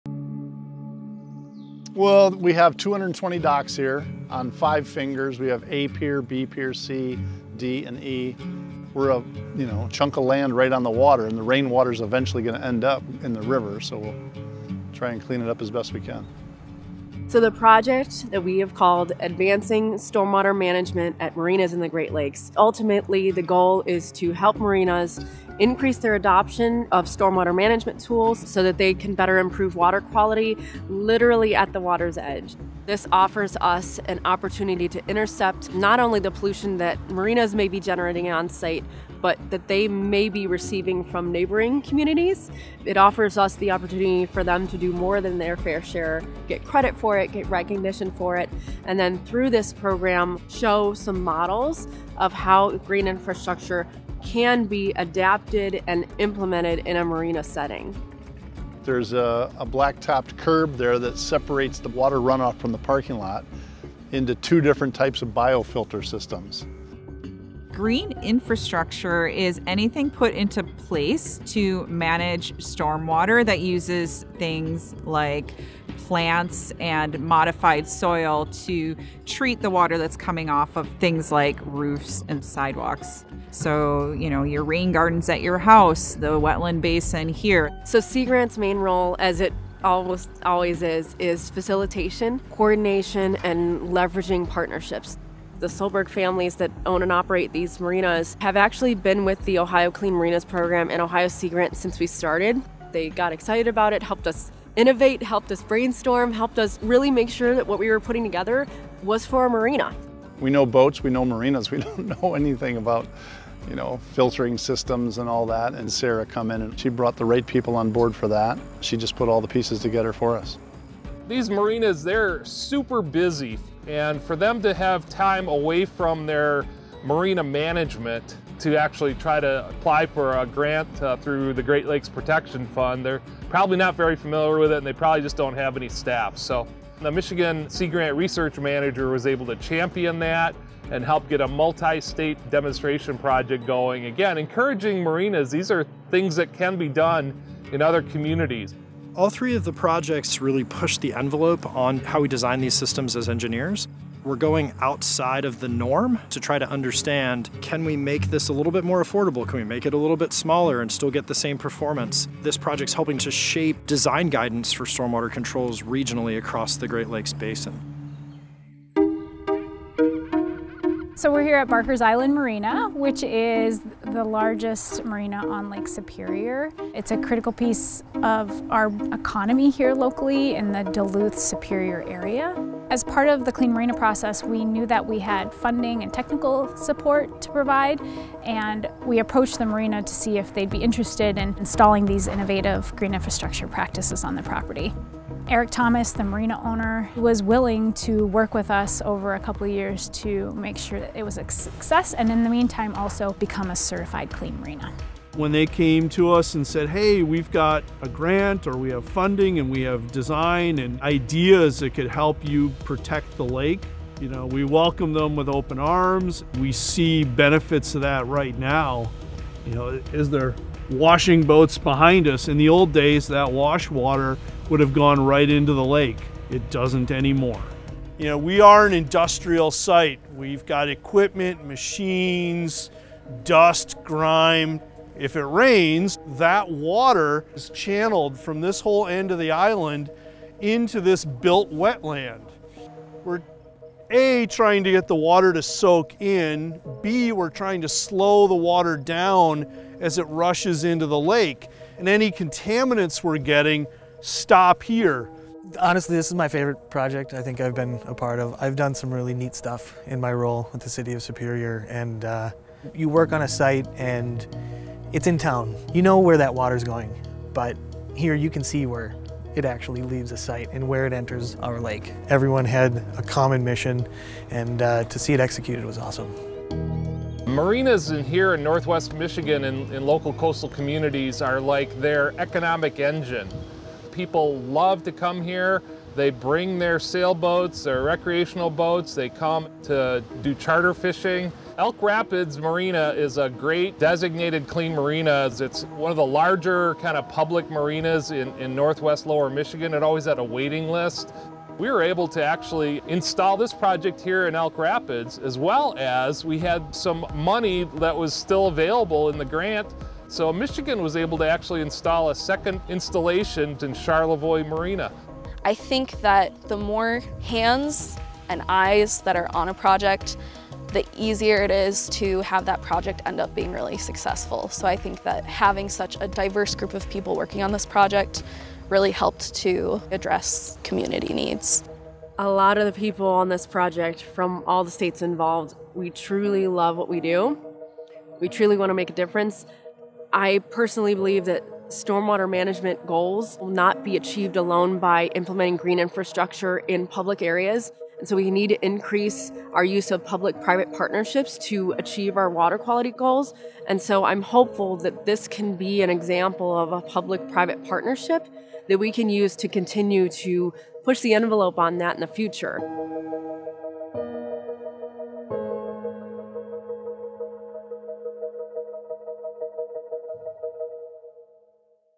Green Infrastructure and Great Lakes Marinas: A Collaboration
Marinas offer a “last chance” opportunity to treat and manage stormwater runoff before it enters the Great Lakes. Nature-based techniques, like green infrastructure, can manage stormwater at your marina, minimize water pollution, eliminate nuisance flooding, and improve aesthetics.

A team from the Michigan, Ohio, and Wisconsin Sea Grant programs, as well as the Ohio State University, are part of a three-year project to help marinas make their facilities more environmentally sustainable by harnessing the power of green infrastructure (GI).

This project team was awarded funds in 2018 by the Great Lakes Protection Fund, an endowment established by seven states in 1989 to support projects that restore or enhance Great Lakes ecosystem health.